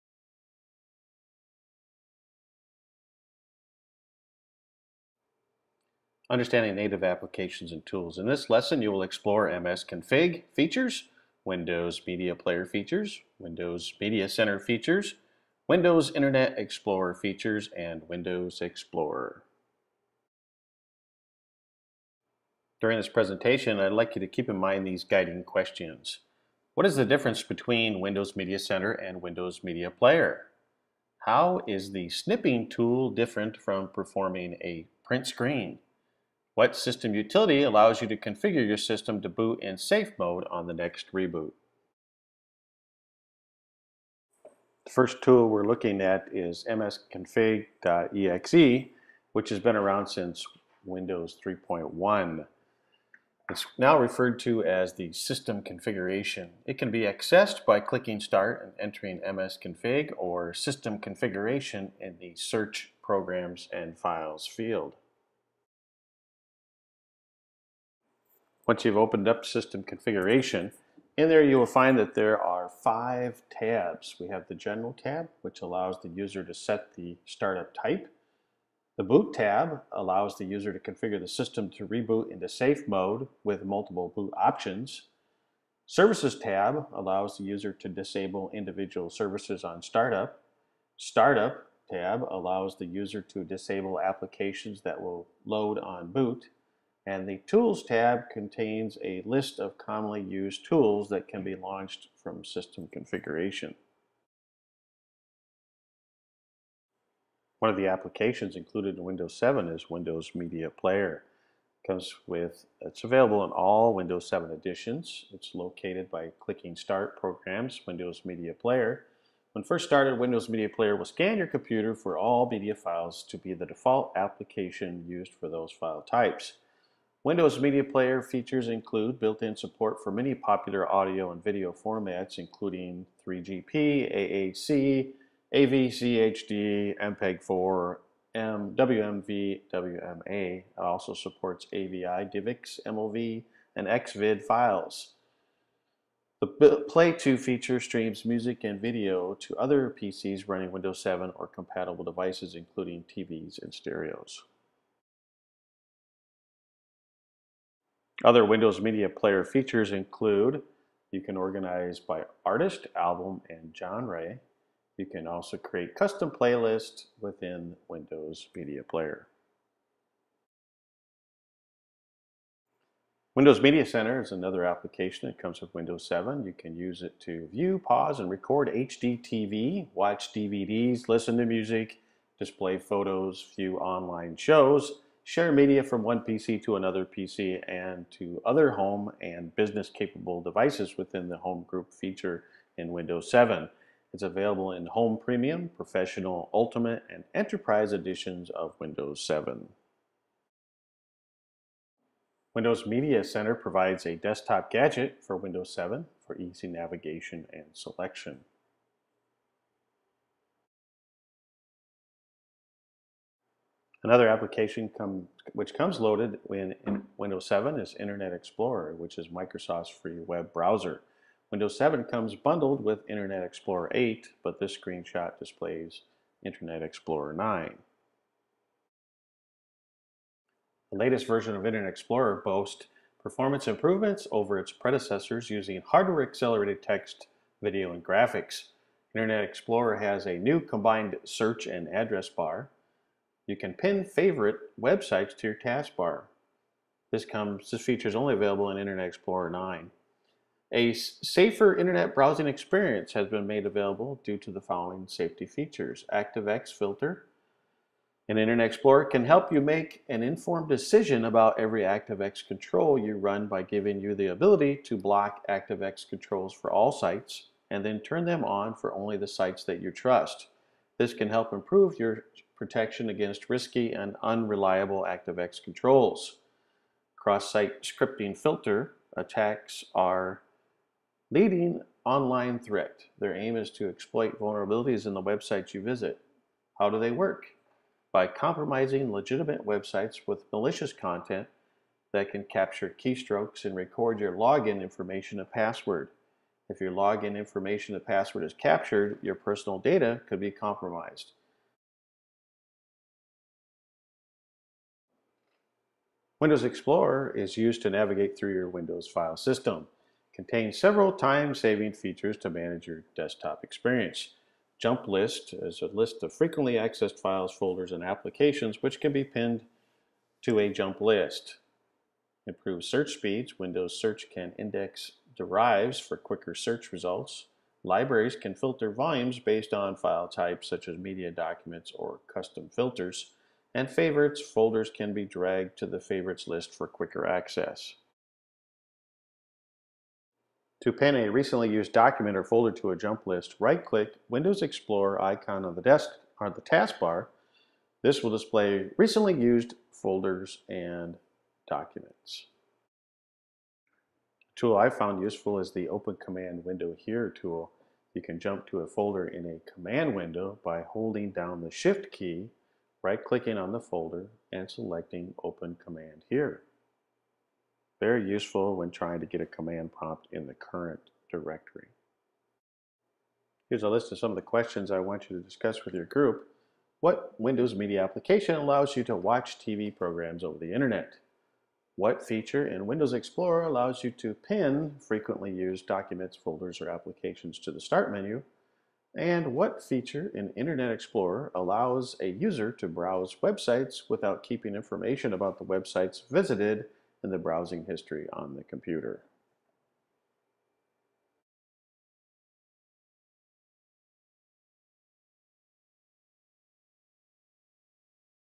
Understand Native Applications and Tools
Understanding Windows Internet Explorer, the snipping tool, Windows Media Player, Windows Media Center and MSCONFIG